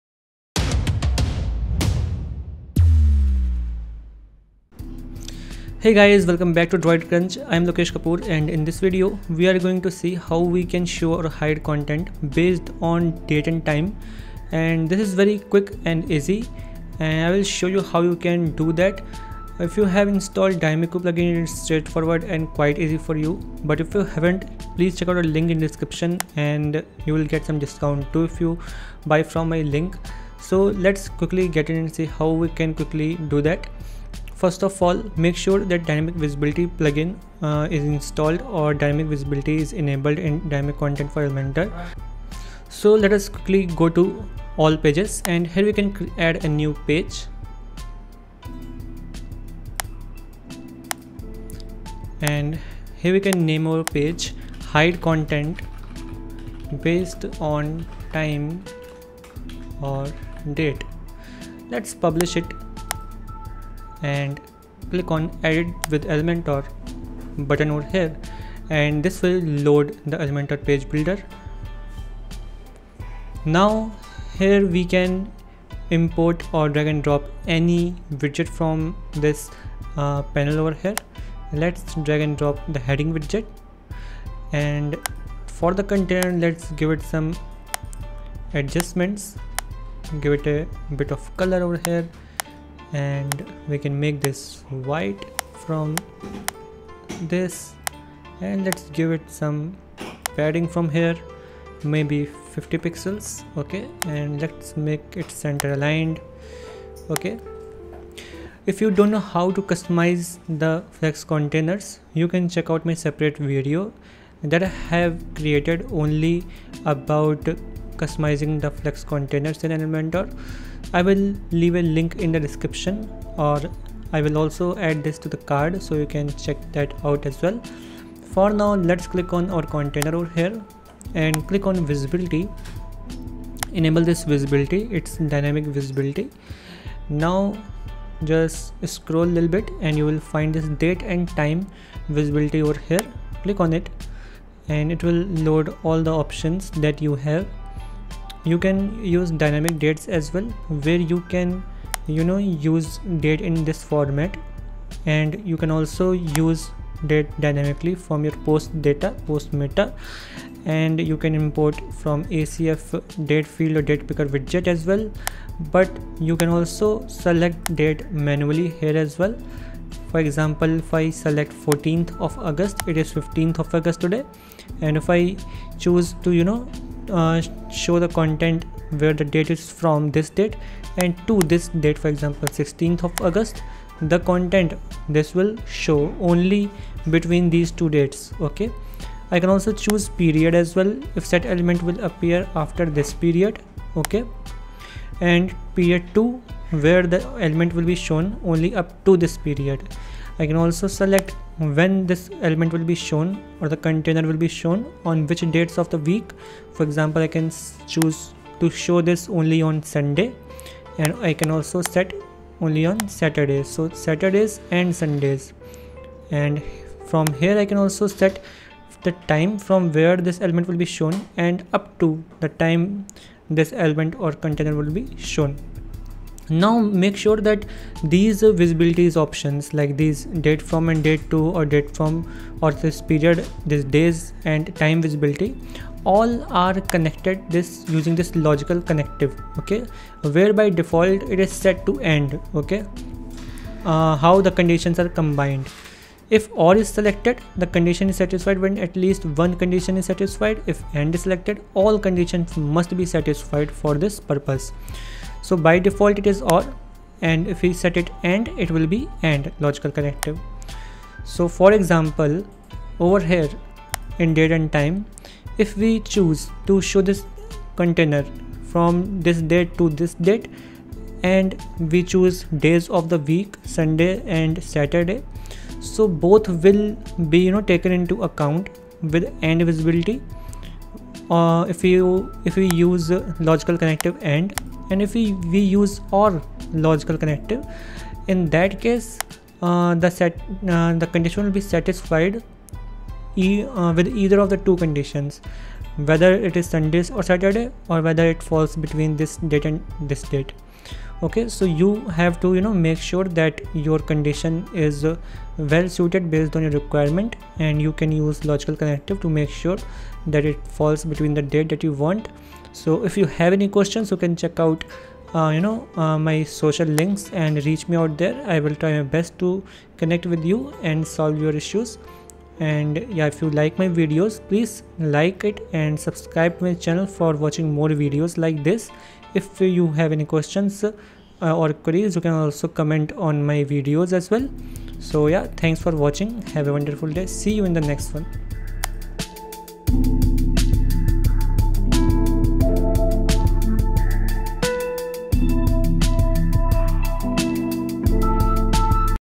Show/Hide Content Based on Data & Time in WordPress🔥
Learn how to show or hide content based on date & time in WordPress using elementor and dynamic content for elementor plugin. By following this tutorial, you will be able to easily hide the content on specific dates, days of week or after a certain date or time. It truly unlocks lots of possibilities.

You can use this visibility feature in your landing pages, offer pages or occision landing pages.

👉🏻 About: DroidCrunch is your trusted blog & YouTube channel dedicated to SAAS lovers, WordPress enthusiasts, and Technology Freaks.